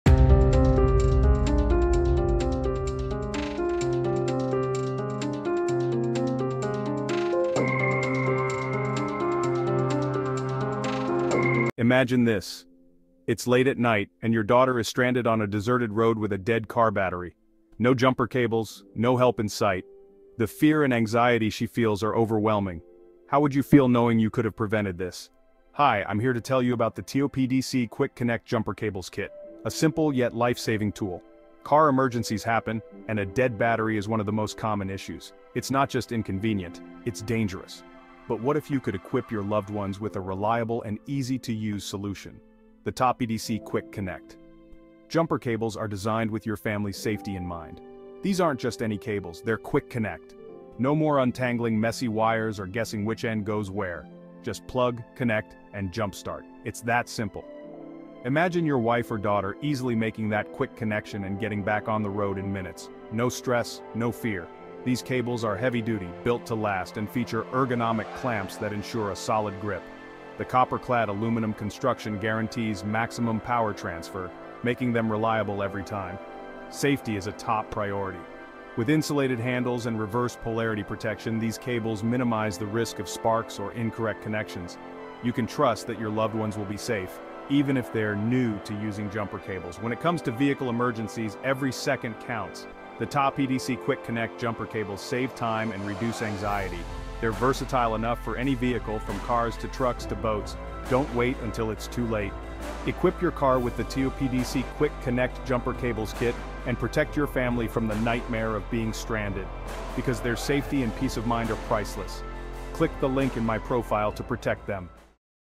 Quick Connect Battery Jumper Cables Kit
What You'll Learn: In this video, we introduce you to the Quick Connect Battery Jumper Cables Kit—a must-have for every vehicle owner. Learn how to effortlessly connect and jumpstart your car battery, ensuring you're never stuck on the side of the road again. We'll walk you through the benefits of using quick-connect jumper cables, how they save you time, and why they're a reliable choice for emergencies.

Go here to grab a kit to keep you and your family safe.

Key Features:

Quick and easy battery connection
Heavy-duty, tangle-free cables
Safe for various vehicle types